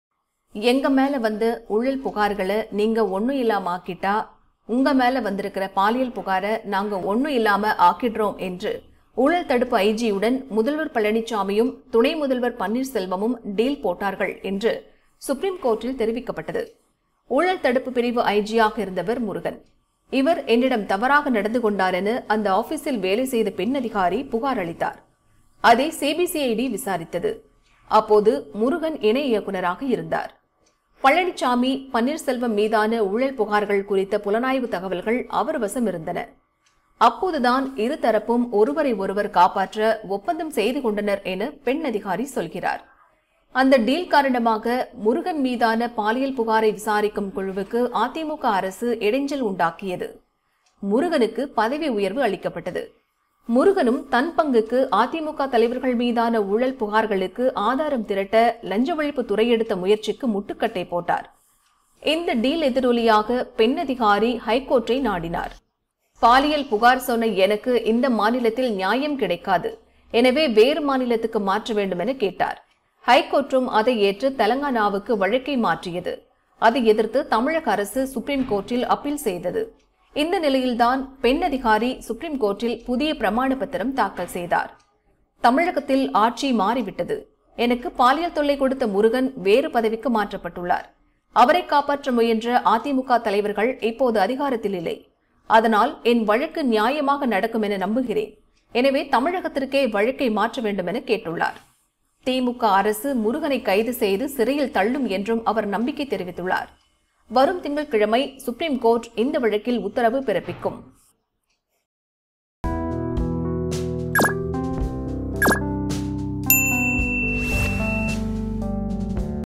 லஞ்ச ஒழிப்பு துறையுடன் டீல் போட்டாரா பழனிசாமி?
எங்க மேல வந்த ஊழல் புகார்கள
நீங்க ஒண்ணும் இல்லாம ஆக்கிட்டா,

உங்க மேல வந்திருக்ற பாலியல் புகார
நாங்க ஒண்ணும் இல்லாம ஆக்கிட்றோம்”

என்று ஊழல் தடுப்பு ஐ.ஜி.யுடன்

முதல்வர் பழனிசாமியும்
துணை முதல்வர் பன்னீர்செல்வமும்

டீல் போட்டார்கள்!”